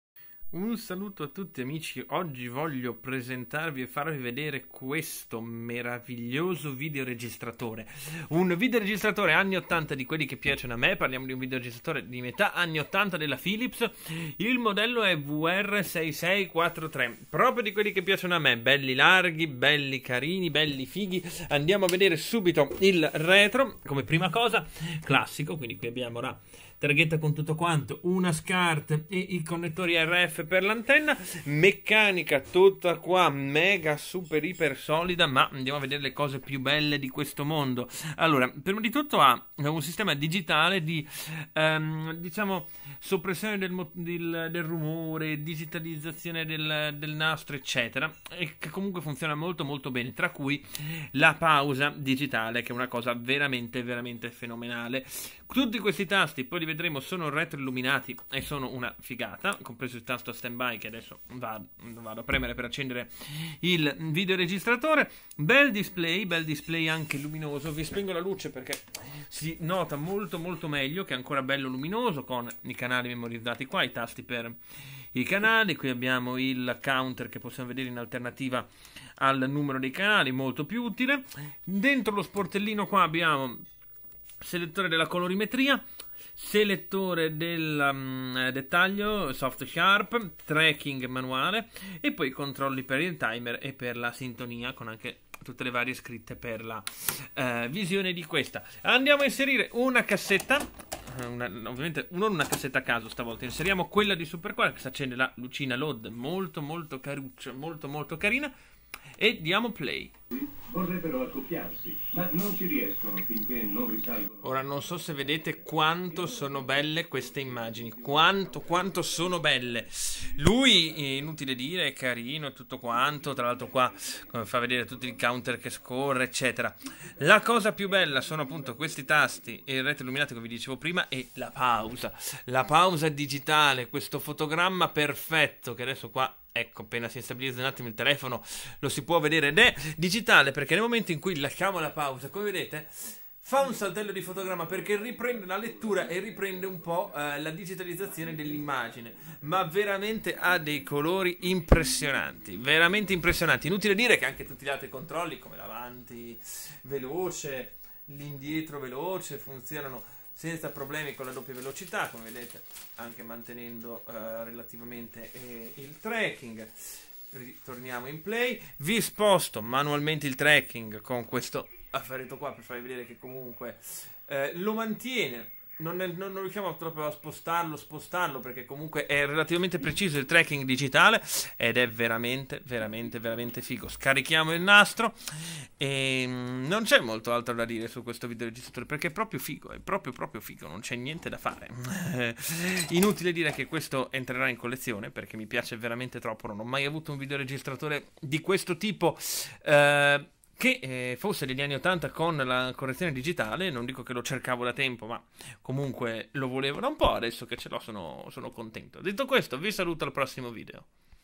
Videoregistratore Philips VR6643 Digital
bellissimo VCR Philips anni '80 di quelli che io AMO alla follia! parliamo di un modello a due testine mono ma che grazie alla correzione digitale dell'immagine prende tutta un altra forma!